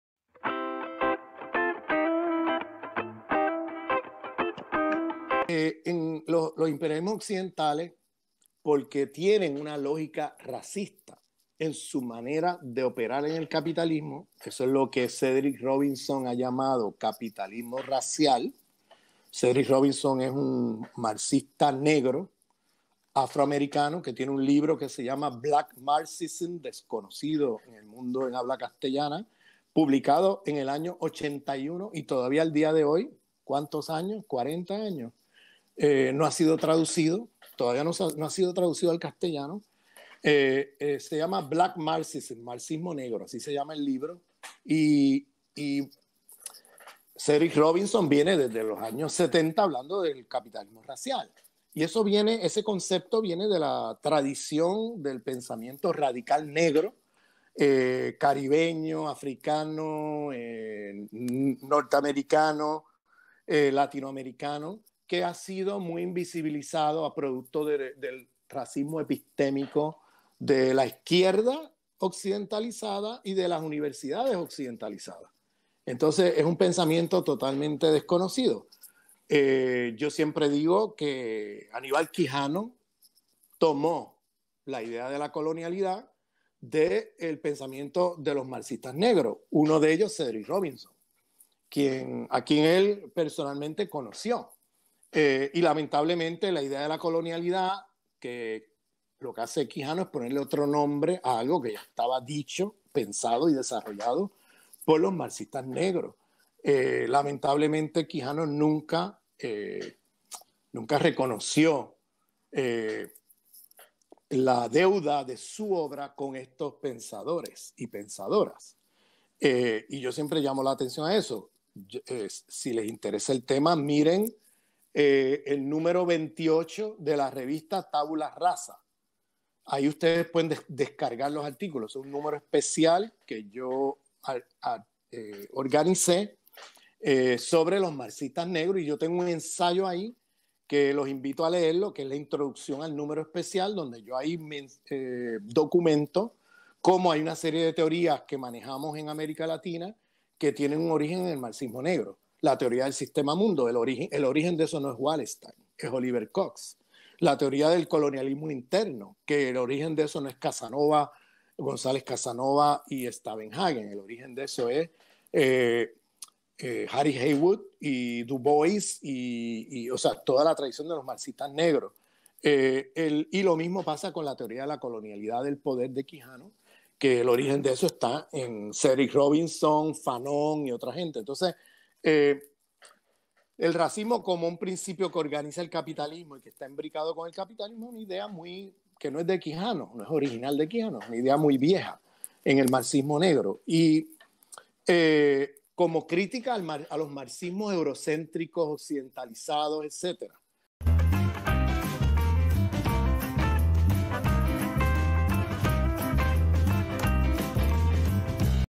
El pensamento de los marxistas negros: “capitalismo y racismo” - Ramón Grosfoguel
La deuda con los aportes del pensamiento crítico, radical, de los marxistas negros, caribeños, africanos, centroamericanos, latinoamericanos: “capitalismo y racismo”, “sistema mundo”, “colonialidad del poder”, “colonialismo interno”.

Este video es parte de la charla con Ramón Grosfoguel (sociólogo, perteneciente al grupo de Modernidad / Colonialidad de la Universidad de Berkley California) en la serie: Diálogos en medio de la pandemia.